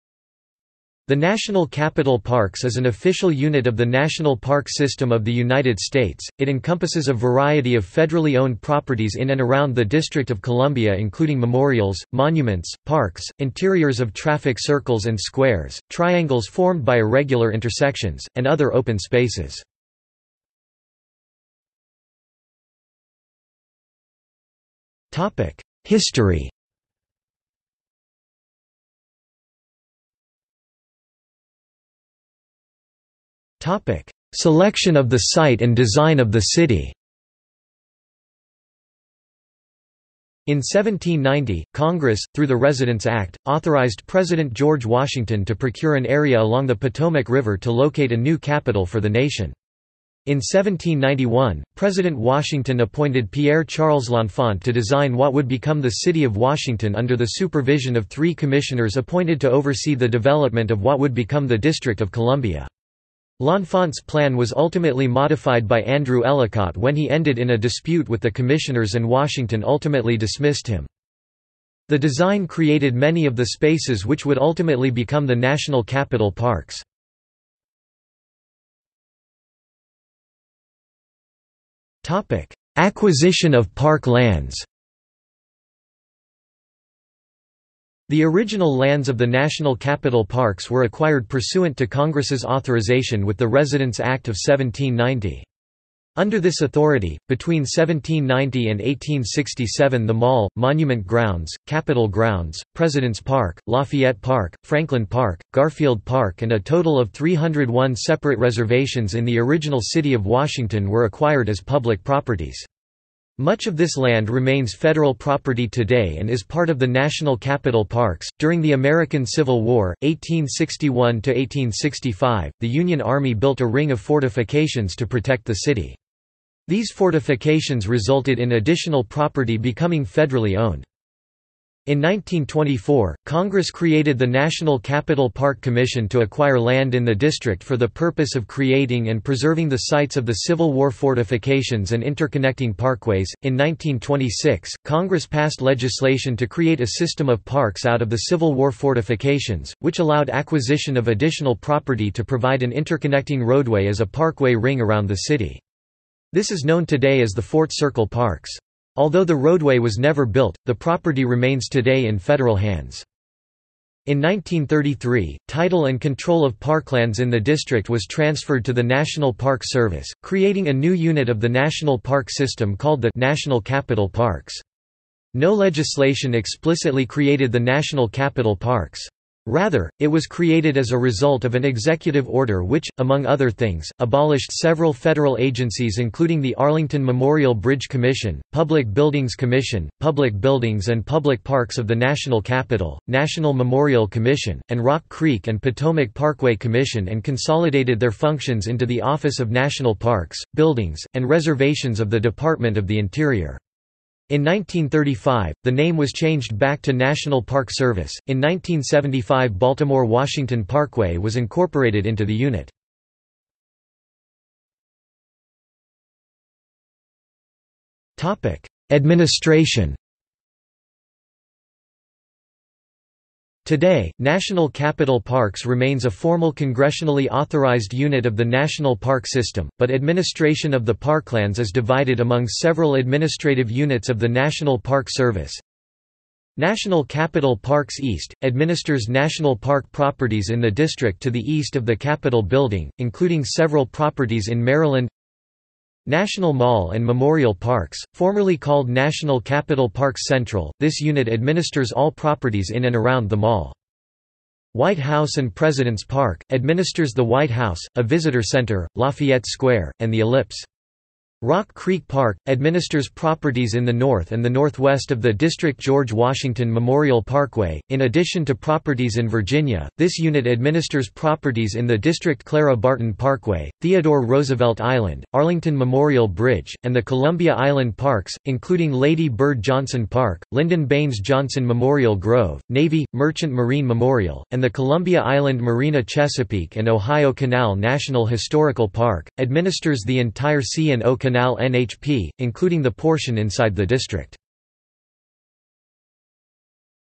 National Capital Parks | Wikipedia audio article
This is an audio version of the Wikipedia Article:
National Capital Parks

00:00:21 1 History
00:00:30 1.1 Selection of the Site and Design of the City
00:01:18 1.2 Acquisition of Park Lands
00:03:34 2 Administration

- Socrates

SUMMARY
The National Capital Parks is an official unit of the National Park System of the United States.  It encompasses a variety of federally owned properties in and around the District of Columbia including memorials, monuments, parks, interiors of traffic circles and squares, triangles formed by irregular intersections, and other open spaces.